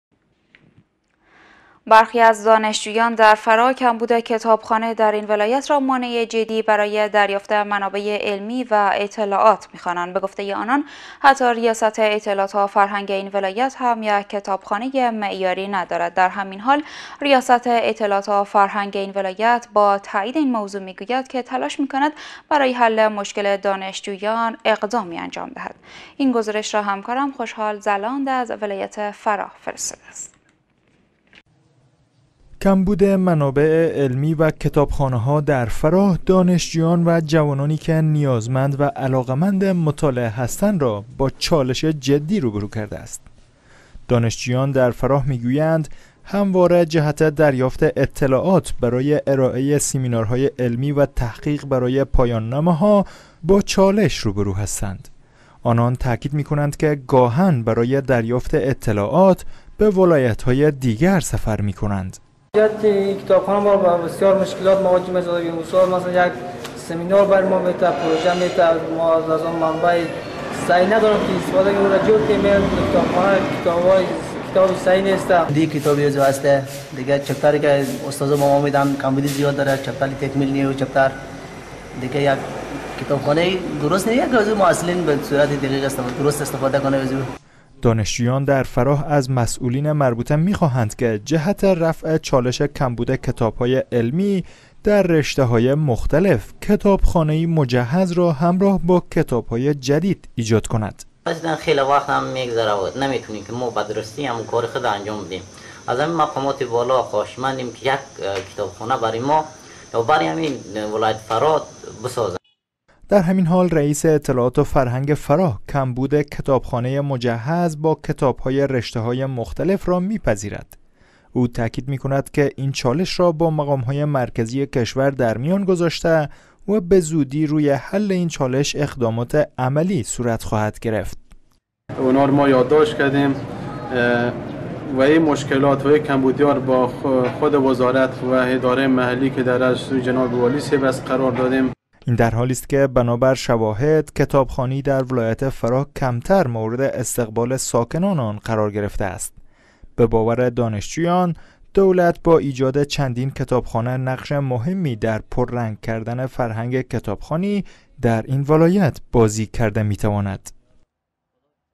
شکایت دانشجویان فراهی از نبود کتابخانه معیاری در این ولایت
برخی از دانشجویان در فراه، کمبود کتابخانه در این ولایت را مانع جدی برای دریافت منابع علمی و اطلاعات می‎خوانند.